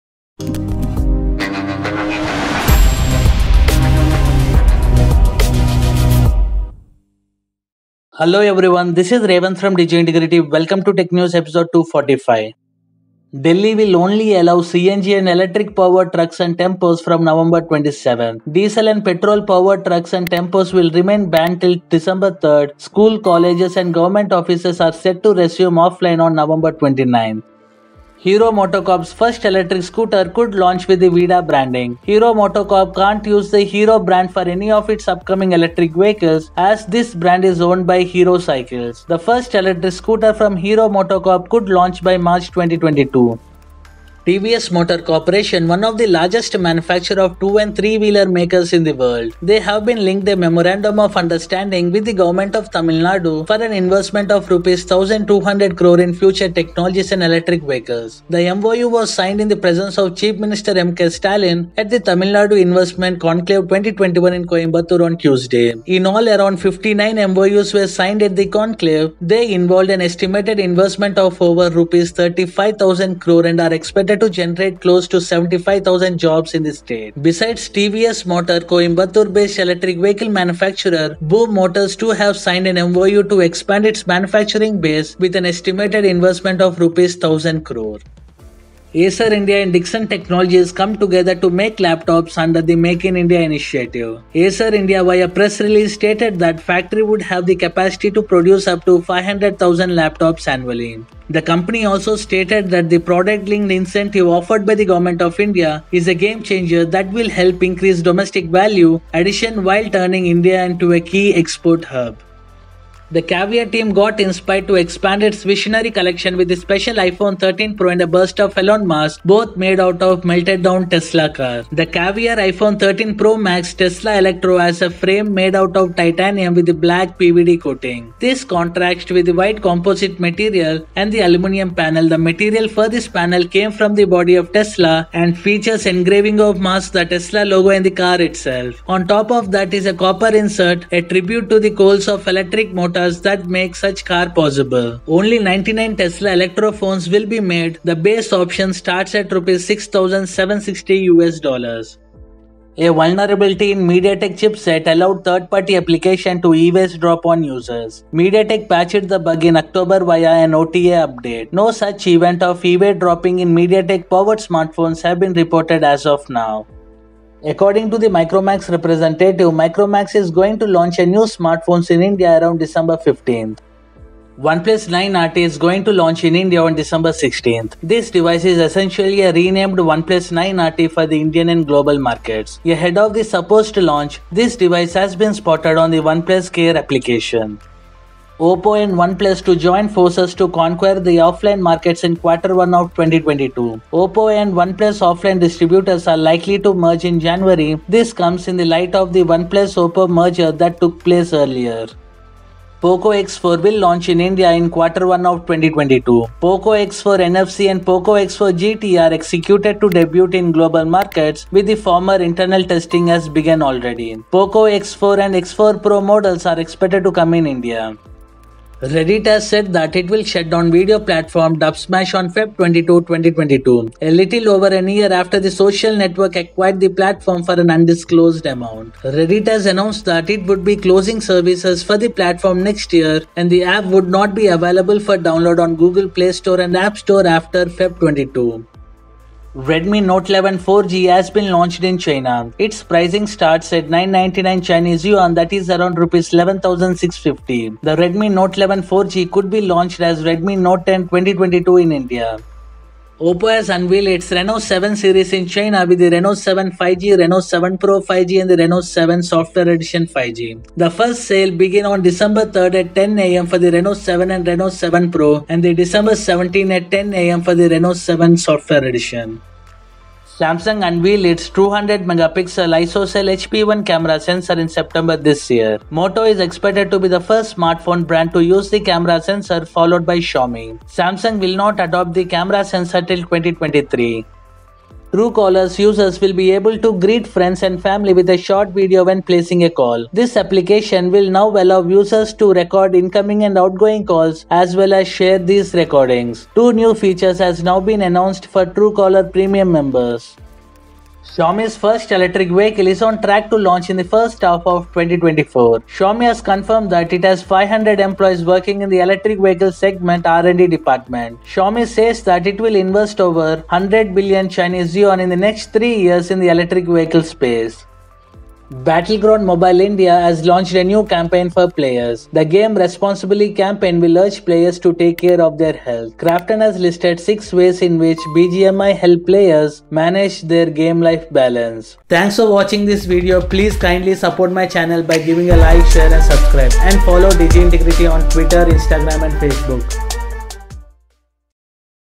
Oppo Reno7 Series,POCO X4,Micromax New Phone,Moto 200MP PhoneTVS Motor to invest Rs 1,200 cr-DIN245
Video Highlights
(0:00) : Introduction
(0:13) : Delhi will allow only CNG and electric vehicles from November 27th to curb air pollution
(0:28) : Upcoming Hero electric scooters could come under the Vida brand name
(0:44) : TVS Motor to invest Rs 1,200 cr in Future Technologies and EV; signs MoU with TN govt
(1:28) : Acer India and Dixon Technologies Come Together to Make Laptops Under The “Make in India” Initiative
(1:53) : Caviar makes a custom iPhone and an Elon Musk bust out of a melted down Tesla Model 3
(2:32) : MediaTek fixes chipset vulnerability that allowed third-party apps to eavesdrop on users
(2:47) : Micromax is going to launch new Smartphones in India around December 15
(2:54) : OnePlus (9) RT spotted on OnePlus Care app in India, suggesting an imminent launch
(3:08) : OnePlus and OPPO tipped to merge offline distributors in Q1 2022
(3:22) : POCO X4, POCO X4 NFC, POCO X4 Pro, and POCO X4 GT launch timelines revealed
(3:38) : Reddit to shut down Dubsmash on February 22, 2022
(3:59) : Redmi Note 11 4G has been launched in China
(4:13) : Oppo Reno7, Reno7 Pro and Reno7 SE go official
(4:30) : Motorola, Xiaomi phones with 200MP cameras tipped to launch in 2022
(4:45) : Truecaller brings Video caller ID, call recording, and more features to Android users in India
(5:01) : Xiaomi electric car team includes 500 members, first EV launch on track for H1 2024
(5:18) : BGMI urges players to game responsibly with a new campaign focused on better game-life balance
(5:32) : Thanks for watching

Jio lost 19M users 😱, 6G In India😮, OnePlus RT & Buds Z2, Moto G31 Coming, OPPO E Scooter-DIN244